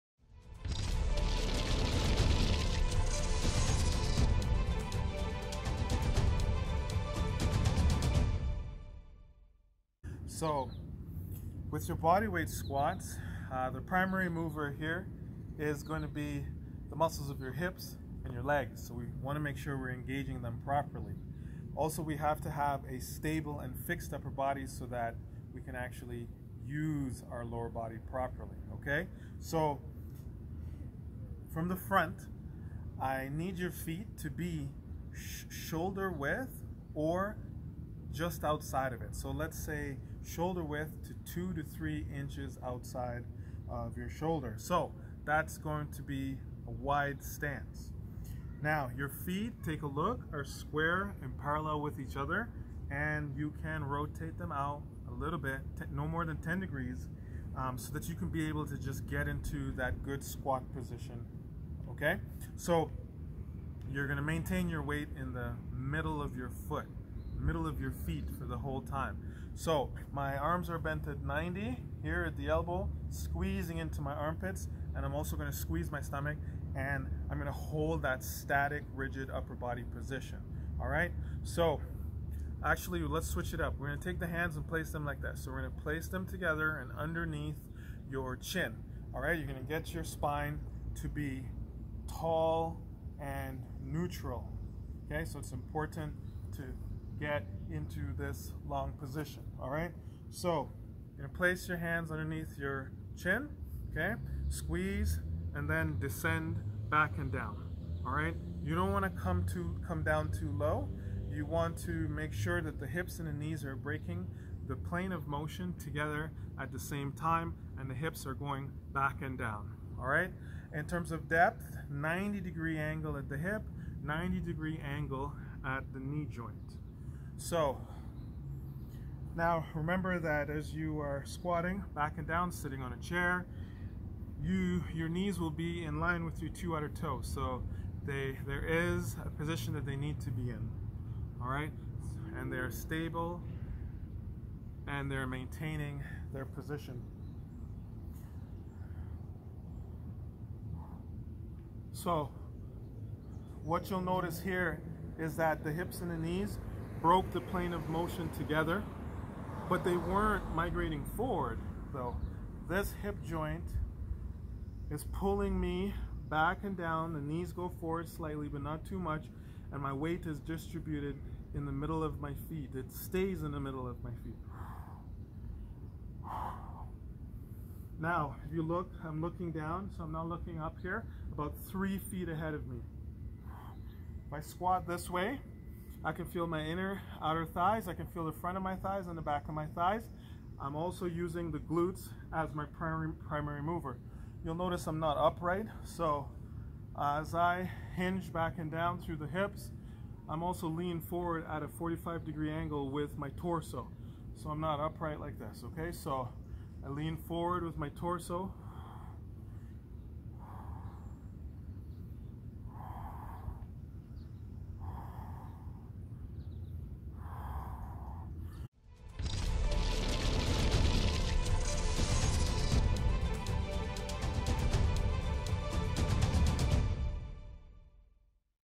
BODYWEIGHT SQUATS (Thumbs up and subscribe if you like this video.)
-Go down slow and hold for 3-sec to 5-sec
-Come up and fully extend your hips by squeezing your glutes
So you think you're a master at squatting at the gym and lunges eh?
Take a look at my cues and technique as they are foundational to squatting with weights, lunges, and movement quality. It will translate to better pain-free movement, increase strength, and lean muscle.

Thanks
CAPT V